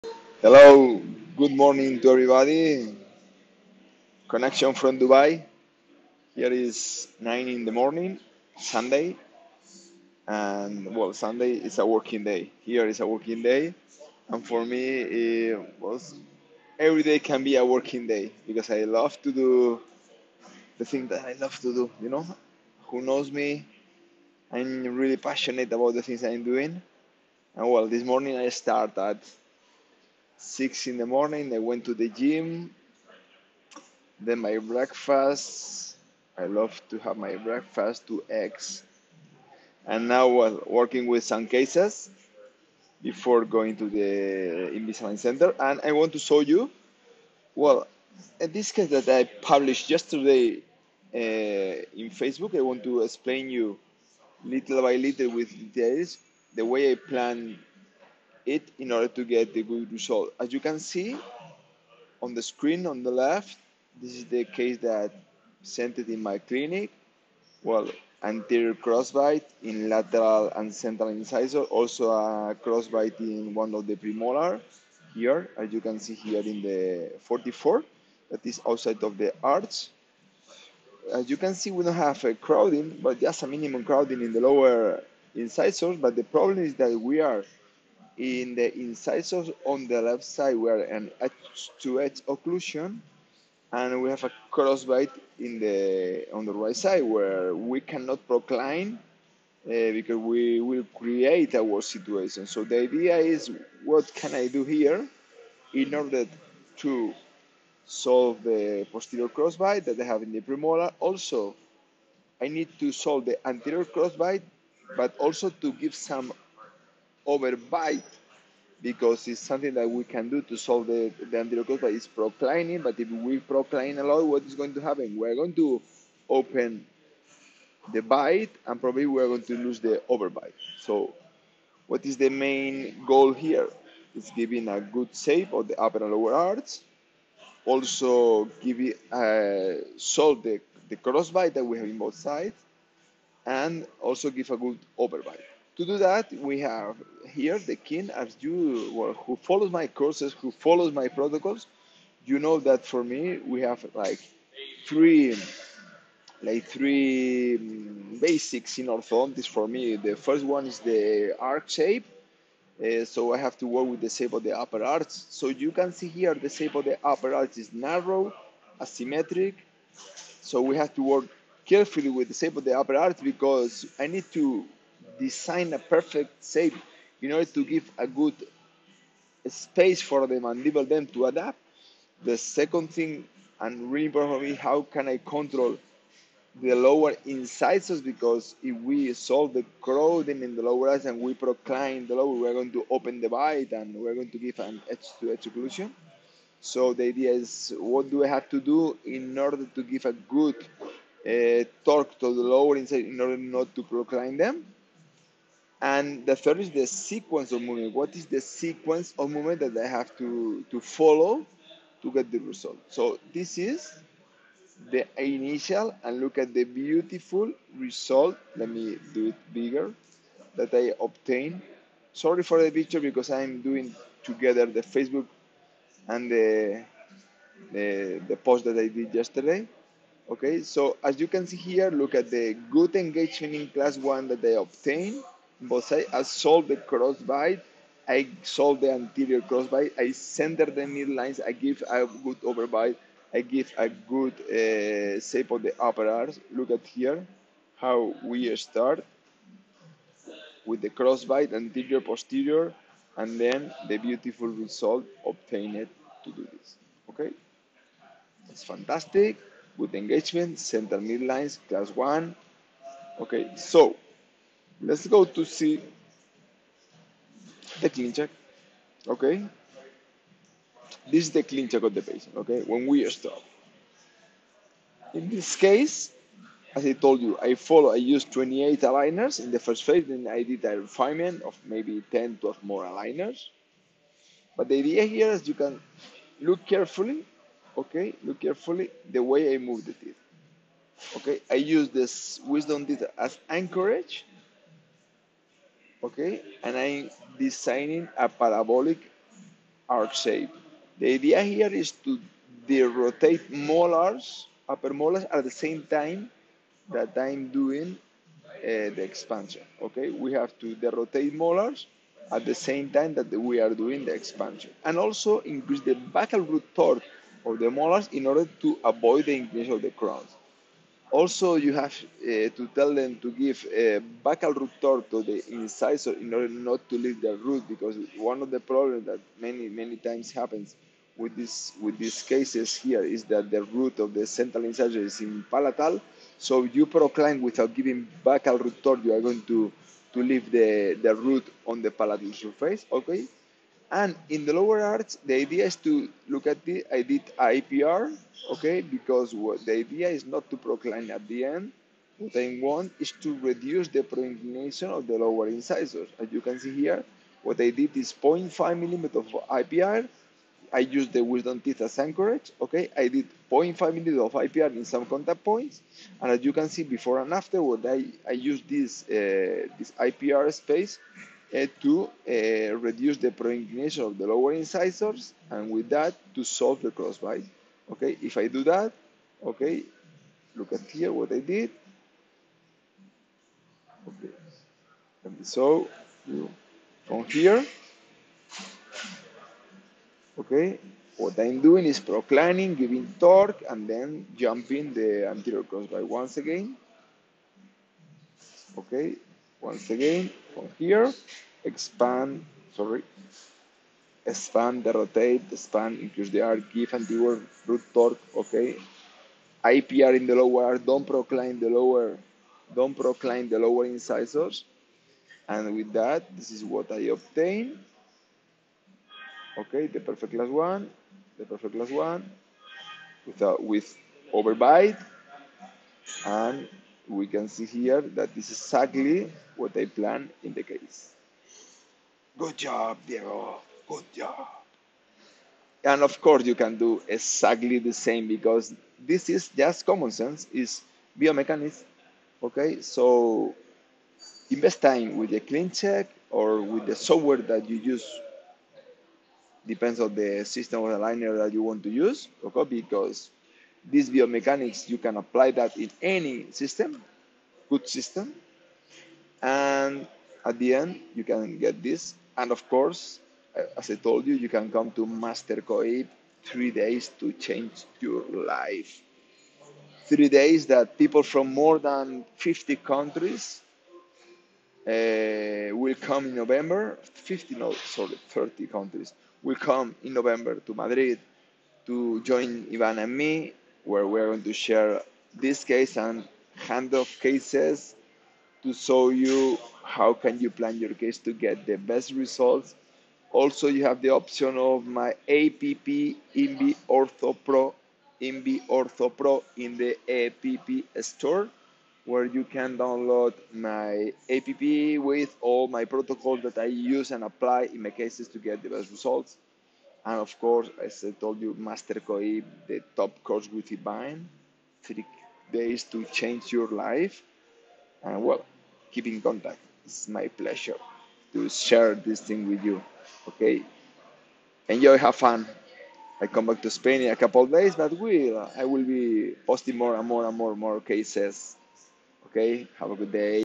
Aligners to solve an anterior crossbite.
In this case I share with you the way a plan the software to get outstanding results using aligners to solve a malocclusion, presenting anterior and posterior crossbite with no overbite.
The top 3 Keys:
- Upper arch development
- Control de Lower incisor position with IPR
- Plan a predictable sequence of movements.

The course that will change your life
and The app from the App Store.
INVIORTHOPRO:
You will find all my protocols